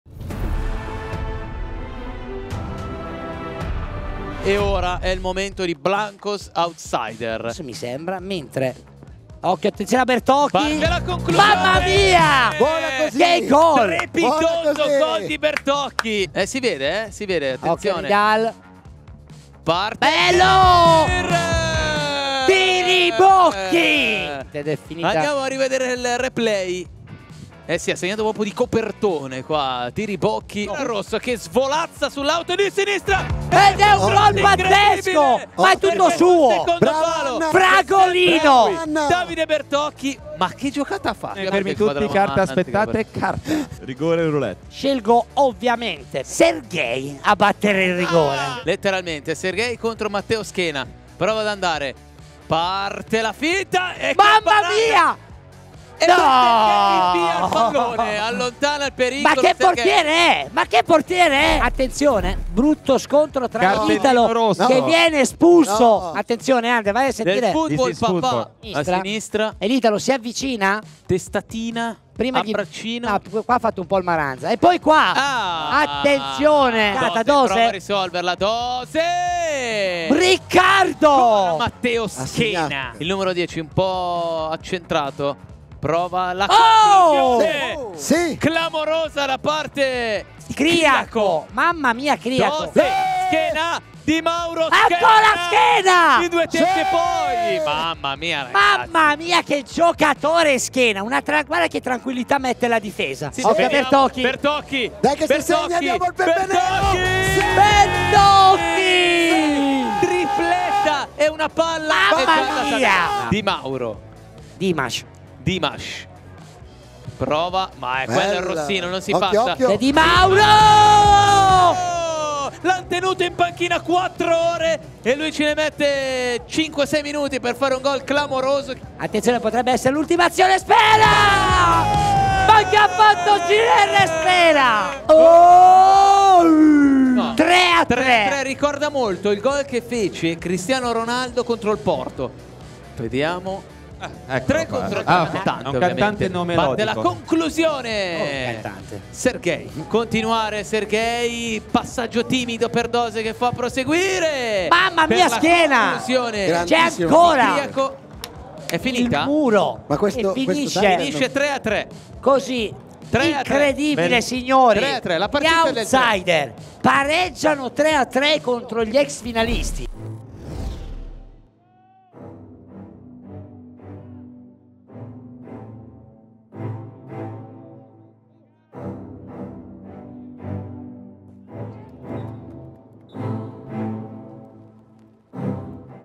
TIRIBOCCHI risponde alla tripletta di BERTOCCHI: Blancos - Outsider | GOA7 League Highlights
GOA7 League Winter Qualifier Girone Bianco - Giorno 1
Blancos - Outsider 3-3